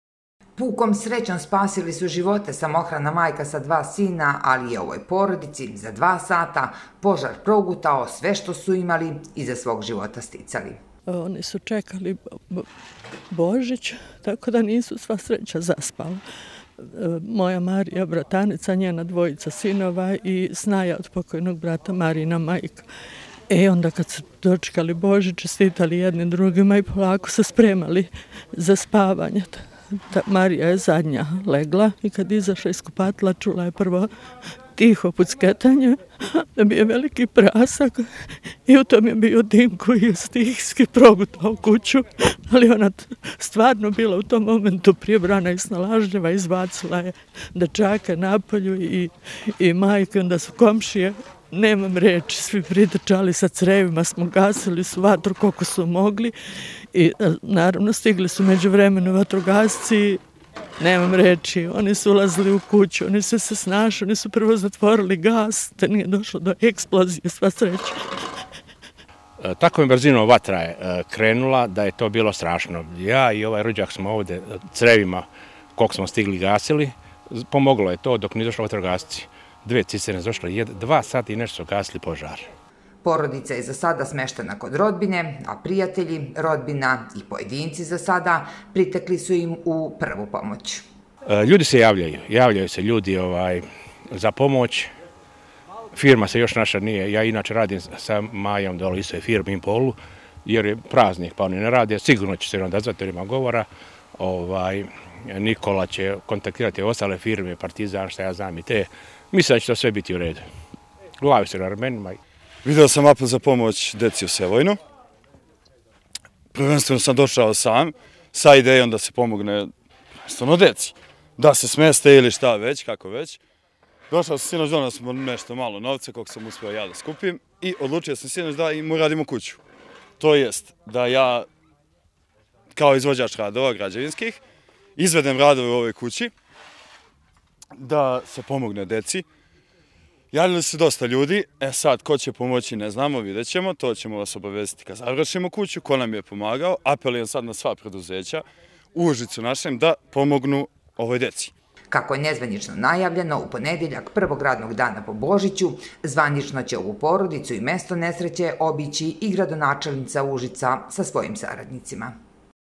08.01.2022. Požar u Sevojnu, izgorela kuća
Kuća porodice Stojić iz Sevojna u kojoj živeli samohrana majka sa maloletna sina u potpunosti je izgorela u stravičnom požaru koji se deseo na Božić. Iako još nema zvaničnog izveštaja pretpostavlja se da su uzrok požara dotrajale instalacije. Samo pukom srećom spaseni su životi dvoje dece i nihove majke, a u ovoj tragerdiji izgubili su celokupnu imovinu. Za sada, pojedinci, rodbina i prijatelji angažovali su se da priteknu ovoj porodici kao prva pomoć, a prema nezvaničnim informacijama, mesto nesreće i ovu porodicu prvog radnog dana po božićnim praznicima obići će i zvaničnici grada Užica.

STUDIO ČAČAK

Adresa: Skadarska 7/3, 32000 Čačak

STUDIO UŽICE